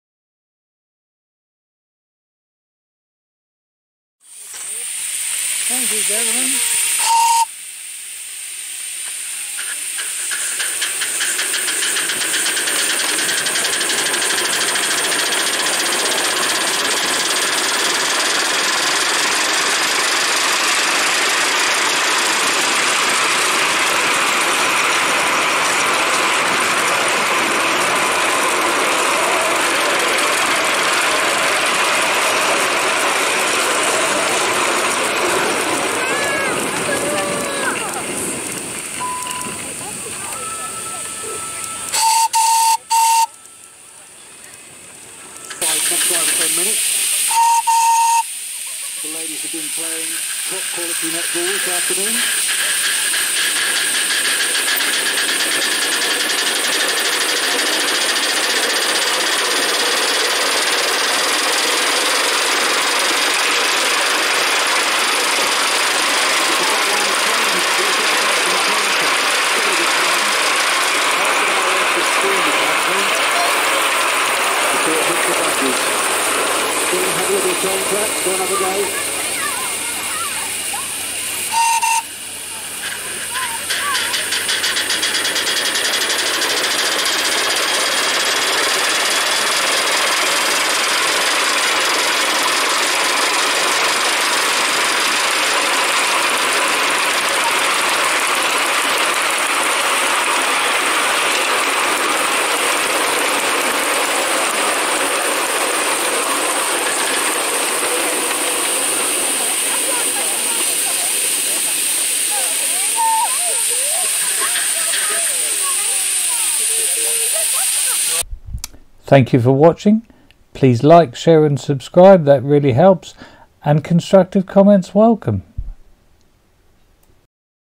0 6 0 Simplex Steam locomotive 5 inch gauge Brian
Here is a Simplex 0-6-0 5" gauge steam locomotive running on our portable track yesterday